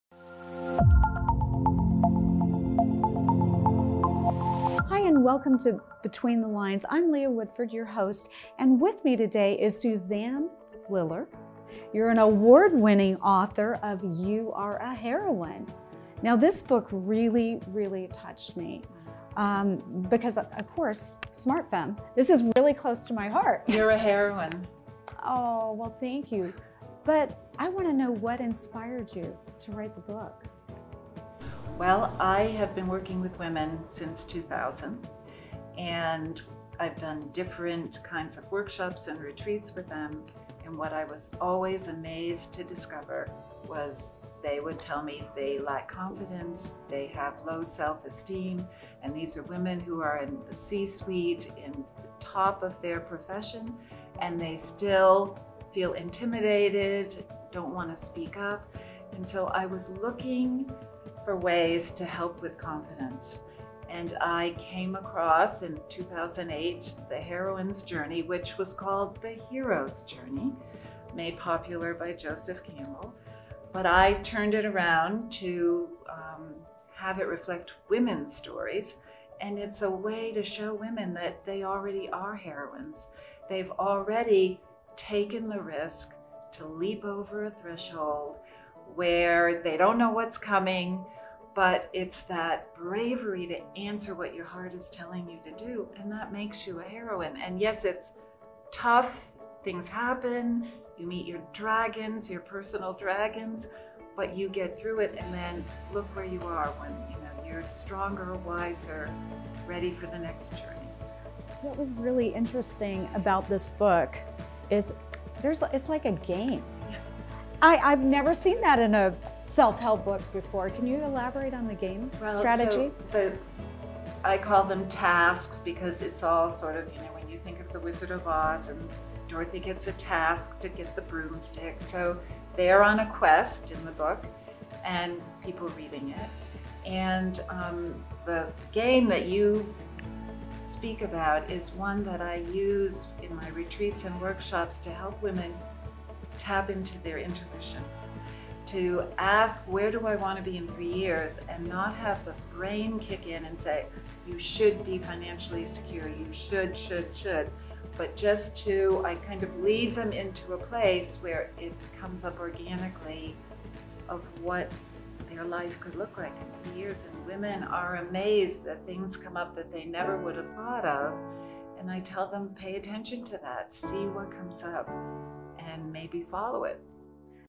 Susanna Liller talks about the Heroines Journey
You are a Heroine encourages women to step in to their greatness and to understand their value.